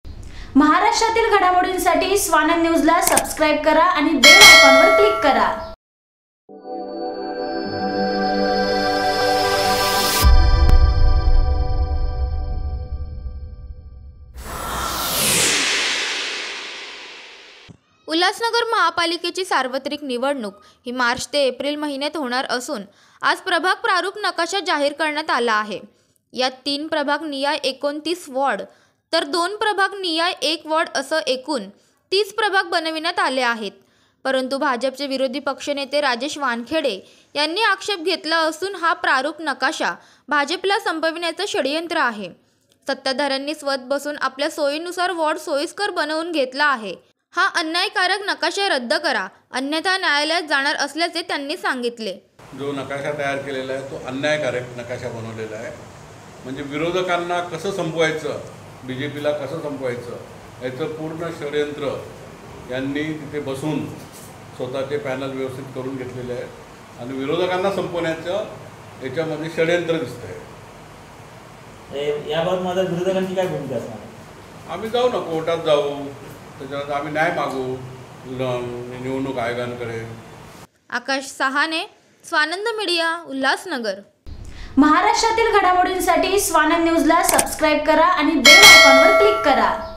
सत्ताधाऱ्यांनी स्वतः बसून प्रभाग प्रारूप नकाशा बनवला आहे - विरोधी पक्षनेता राजेश वानखेडे | उल्हासनगर
SEND ME STUFF:

Near B.K.No.1416,
Maratha section - 32,
Ulhasnagar - 421004,
Dist. : Thane,
Maharashtra,
India